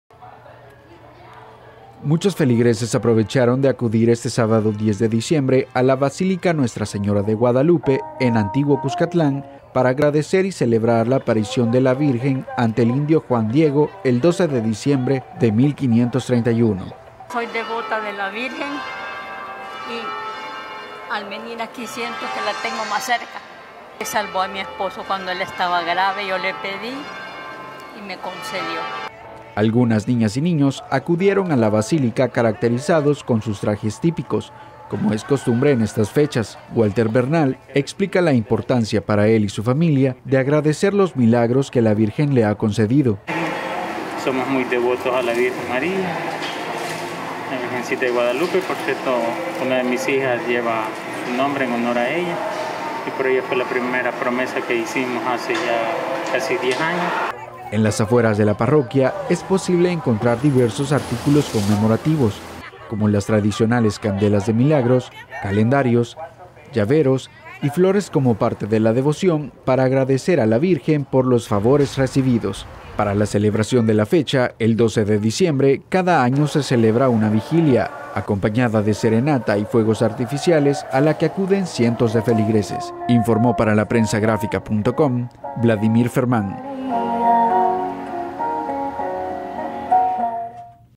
Feligreses acuden a la basílica Nuestra Señora de Guadalupe para agradecer milagros
Muchos feligreses aprovecharon de acudir este sábado diez de diciembre a la Basílica Nuestra Señora de Guadalupe en Antiguo Cuscatlán para agradecer  y celebrar la aparición de la virgen ante el indio Juan Diego el 12 de diciembre de 1531.
¡Visítenos!